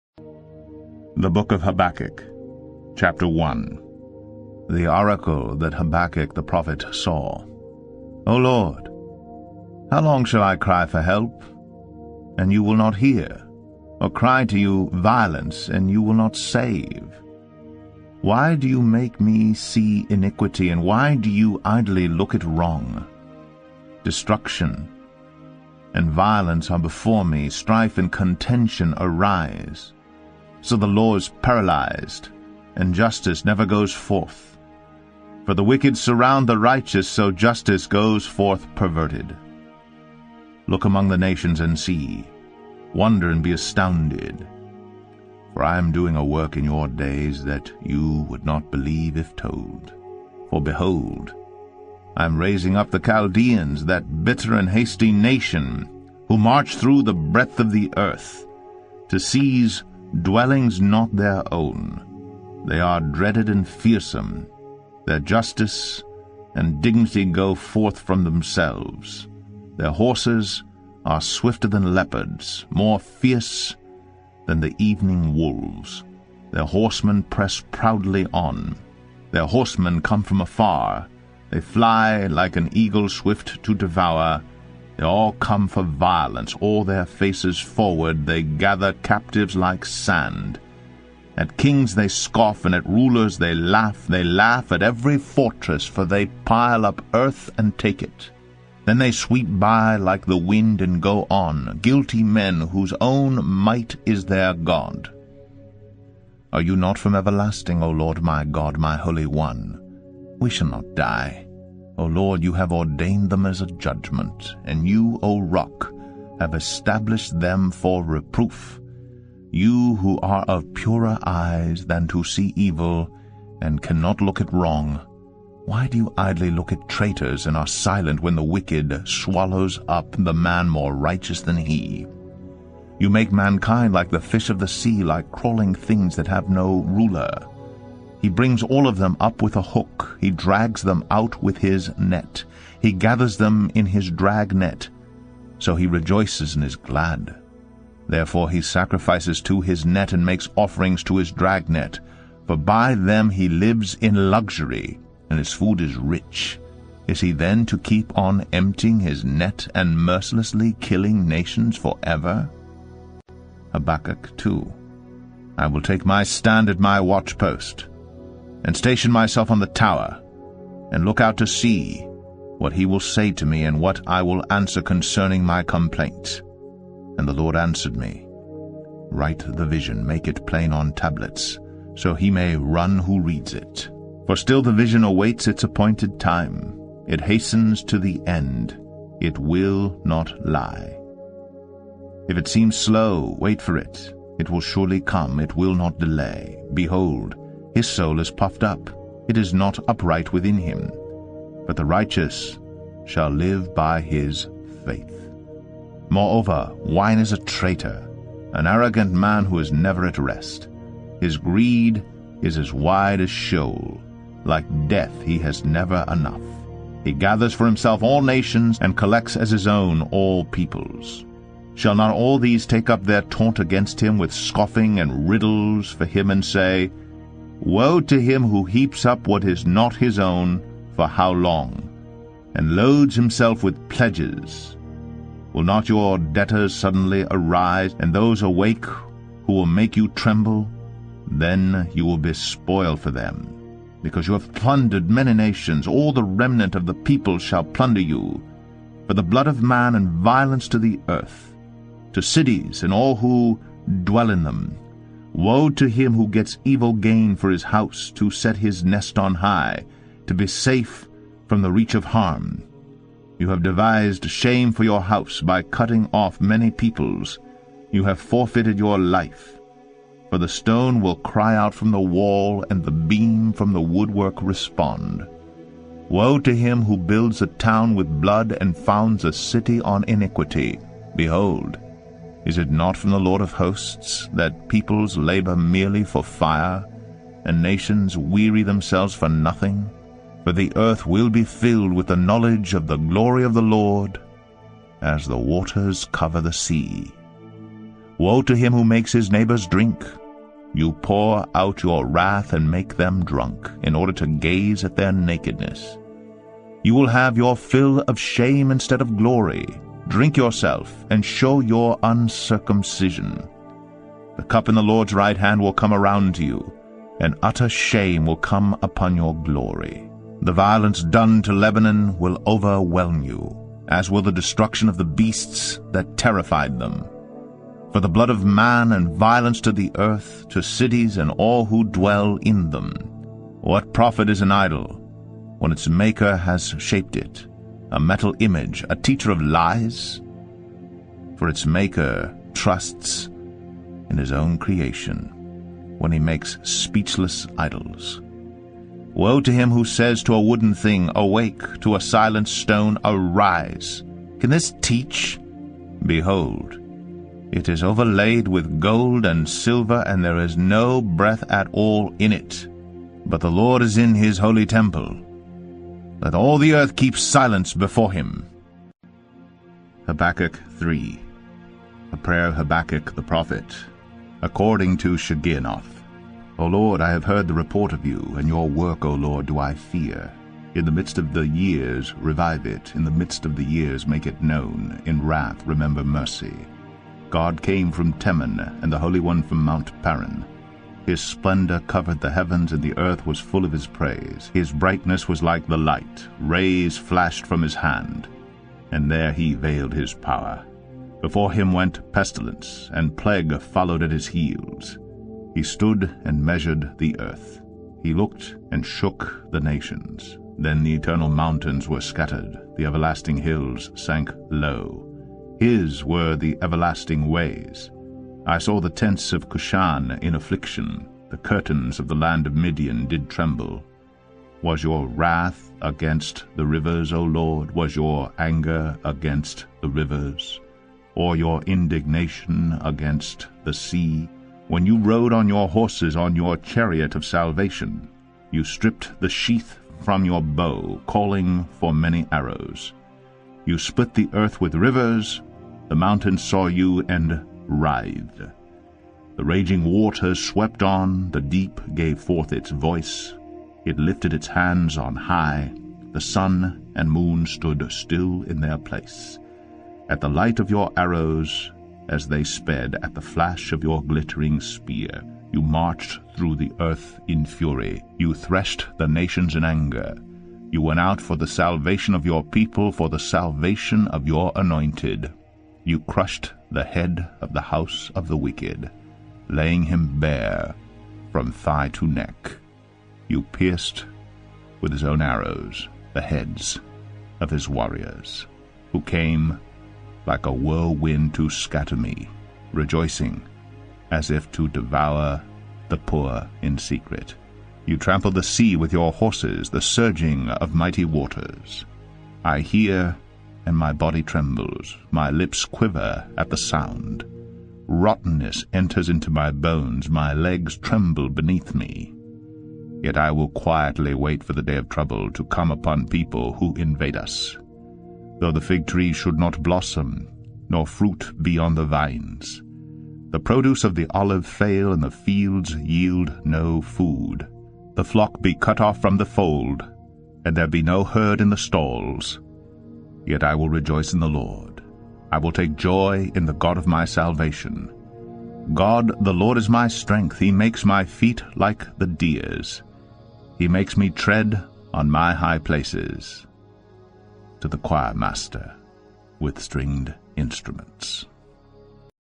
The Book of Habakkuk (ESV) | Full Audio Bible with Text by Max McLean
Brief Summary: The Book of Habakkuk begins with Habakkuk crying out to God for an answer to why God’s chosen people are allowed to suffer in their captivity (Habakkuk 1:1–4). The Lord gives His answer to Habakkuk, essentially stating, “You wouldn’t believe it if I told you” (Habakkuk 1:5–11). Habakkuk then follows up by saying, “OK, you are God, but, still, tell me more about why this is happening” (Habakkuk 1:17—2:1). God then answers him again and gives him more information, then tells the earth to be silent before Him (Habakkuk 2:2–20). Then Habakkuk writes a prayer expressing his strong faith in God even through these trials (Habakkuk 3:1–19).